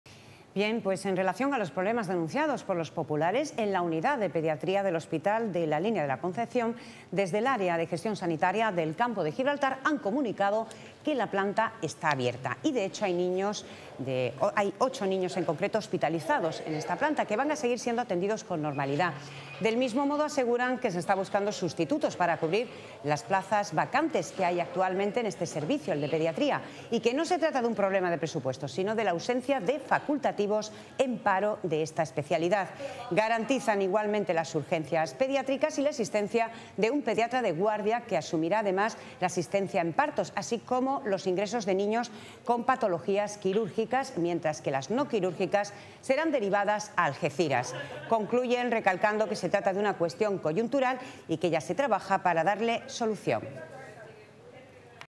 El Área Sanitaria asegura que la planta de pediatría del hospital de La Línea está abierta
En relación a los problemas denunciados por los populares en la unidad de pediatría del hospital de La Línea, desde el Área de Gestión Sanitaria del Campo de Gibraltar han comunicado que la planta está abierta y de hecho, hay 8 niños hospitalizados en ella, que seguirán siendo atendidos con normalidad. Del mismo modo, aseguran que se están buscando sustitutos para cubrir las plazas vacantes que hay actualmente en dicho servicio y que no se trata de un problema de presupuesto, sino de la ausencia de facultativos en paro de esta especialidad. Garantizan igualmente las urgencias pediátricas y la existencia de un pediatra de guardia que asumirá además la asistencia en partos, así como los ingresos de niños con patologías quirúrgicas, mientras que las no quirúrgicas serán derivadas a Algeciras. Concluyen recalcando que se trata de una cuestión coyuntural y que ya se trabaja para darle solución.